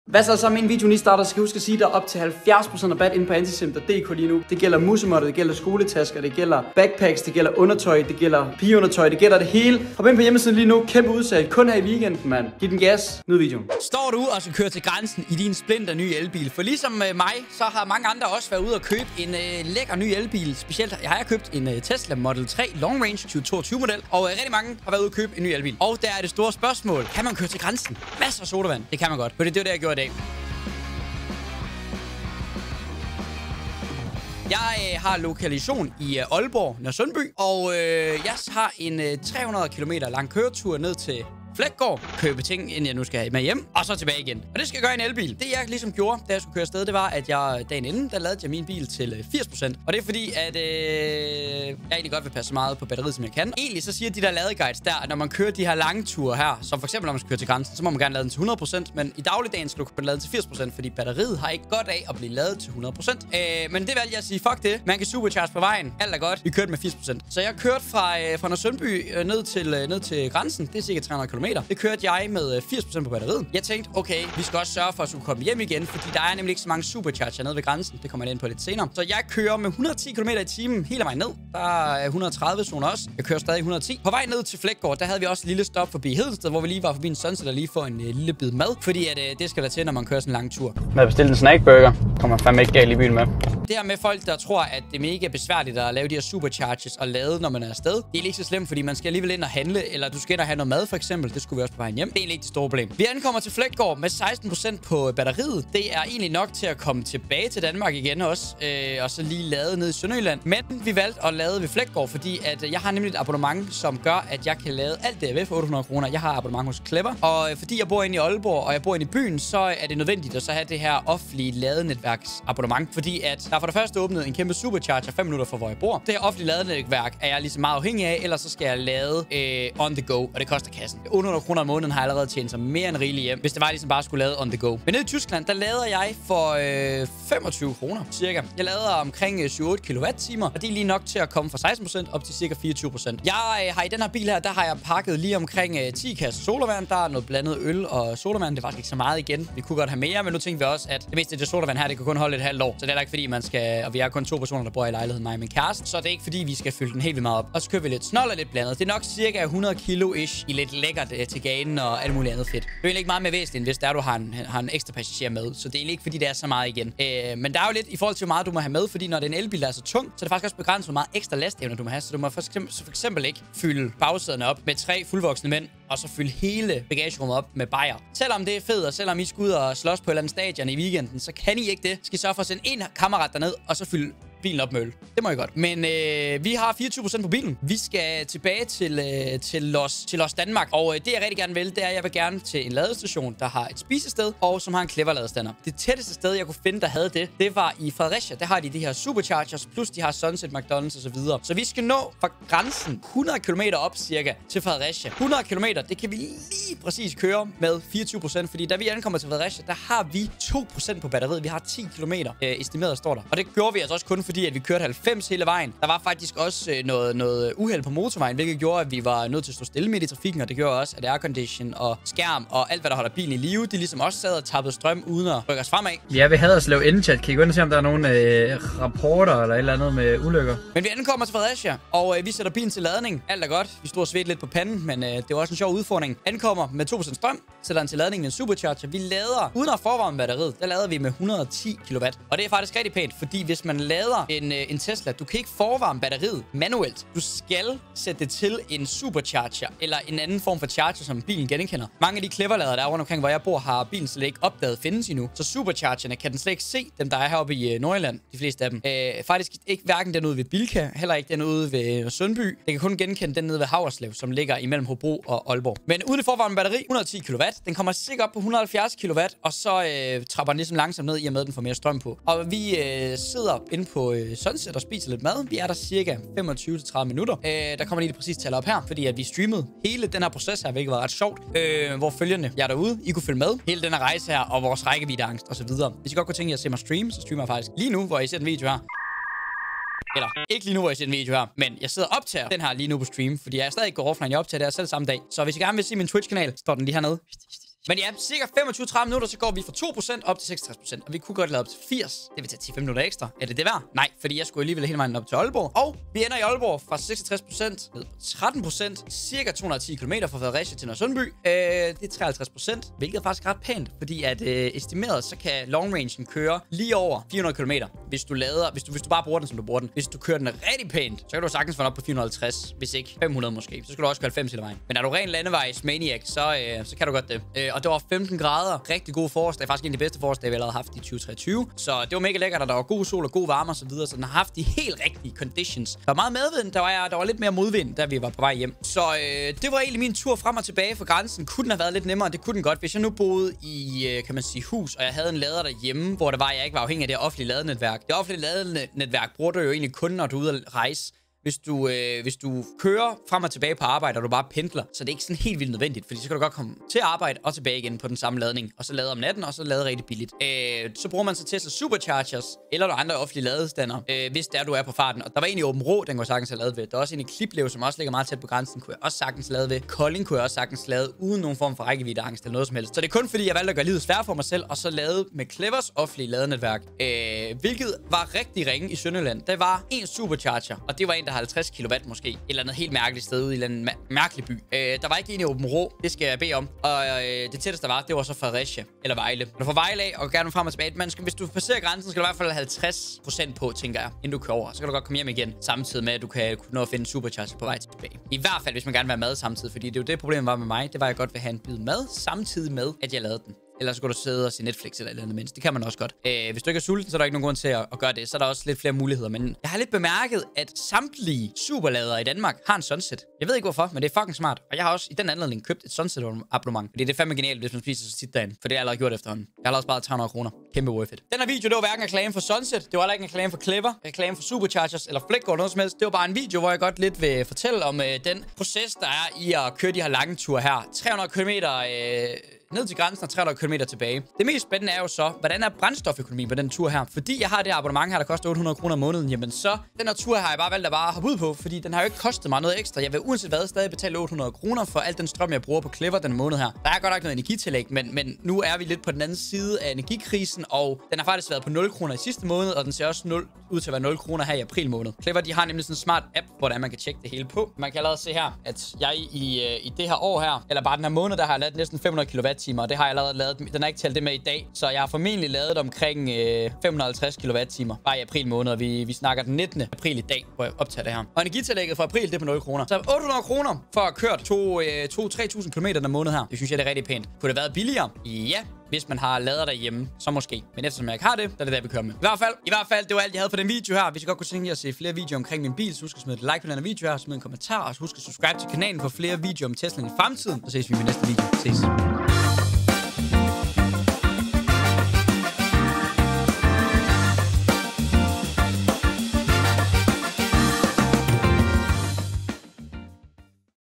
KØRER TIL GRÆNSEN I EN ELBIL (0% VED ANKOMST?)
SPAR OP TIL 70% PÅ ANTISIMP LIGE NU! KUN I WEEKENDEN!

Jeg har for nyligt købt en ny Model 3 Long Range 2022 og har allerede kørt mange kilometer i den! Kæresten og jeg fik en hyggelig ide om at tage til grænsen og handle lidt, nu hvor kørsel i bilen er så billigt! Men hvor besværligt er det at køre til grænsen i en elbil? Hvor nervøs skal man være for at løbe tør for strøm?